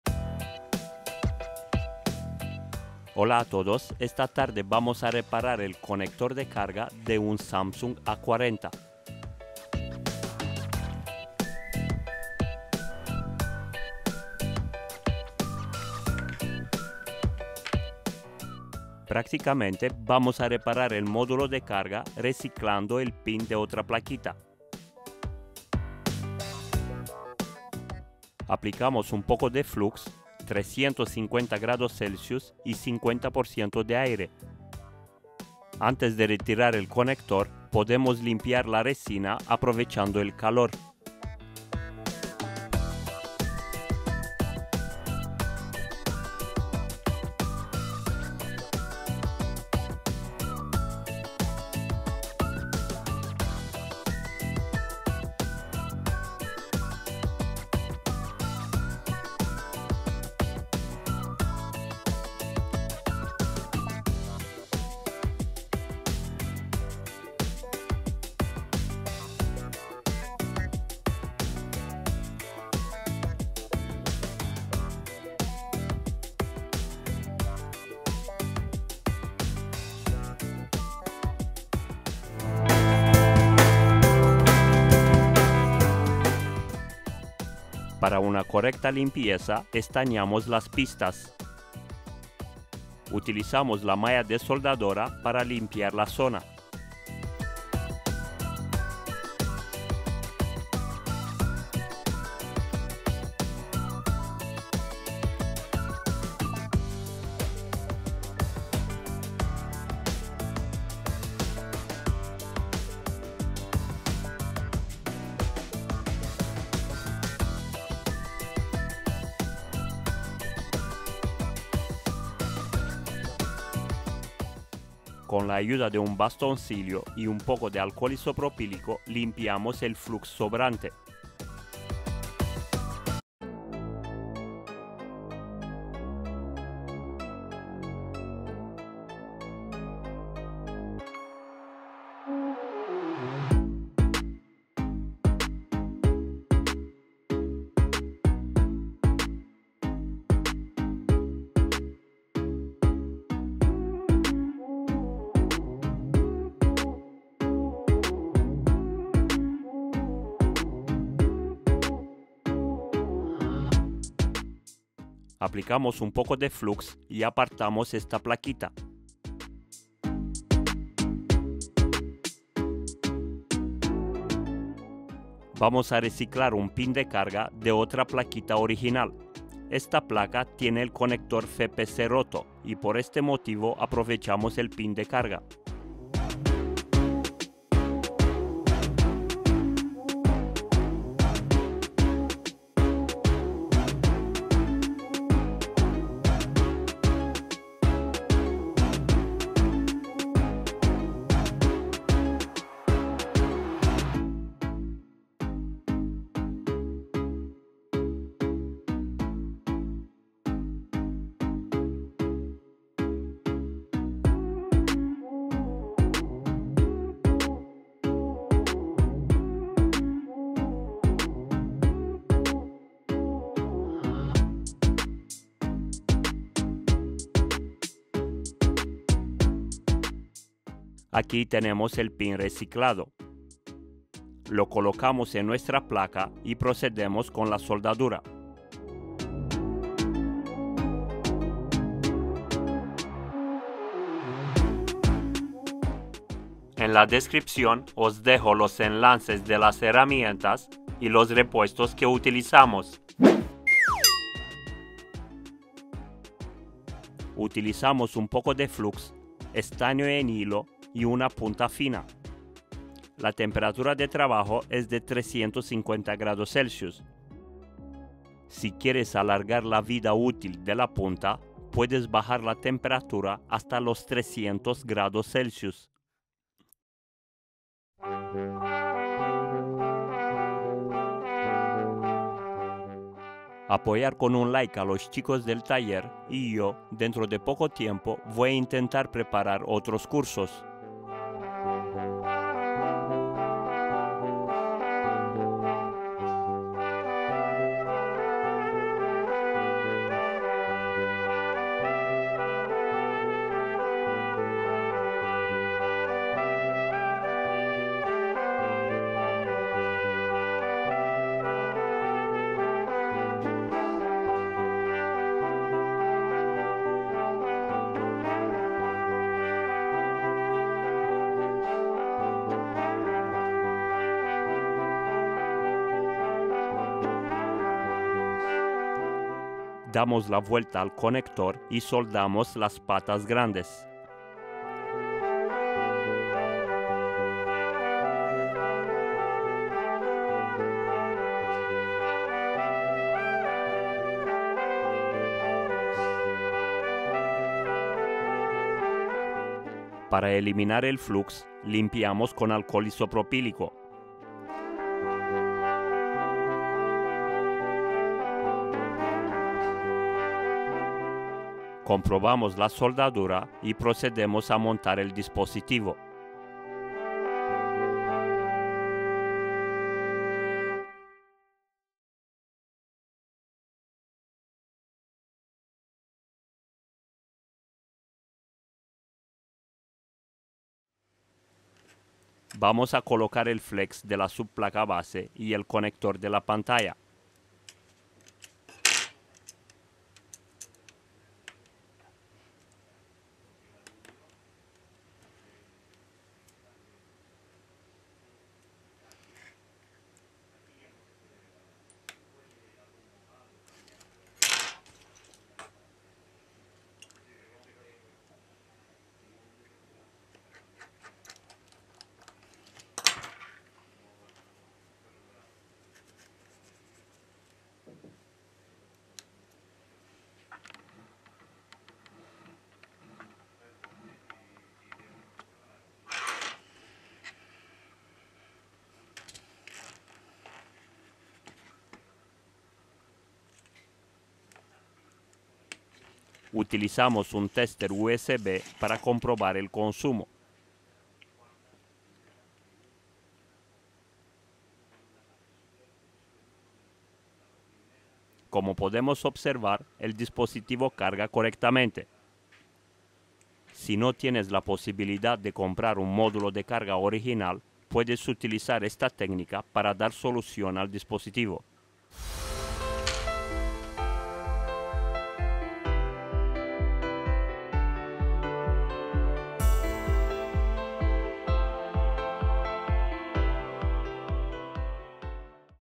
Samsung A40 No Carga Cambiar Pin de Carga
Galaxy A40 reemplazar el pin de carga. Esta tarde vamos a ver cómo podemos reciclar un conector de carga de un modulo de roto. Para este proceso hemos utilizado un soldador a 350ºC y una estación de aire con la misma temperatura y 50% de aire.

🔴 Pin de carga
🟡 Modulo de carga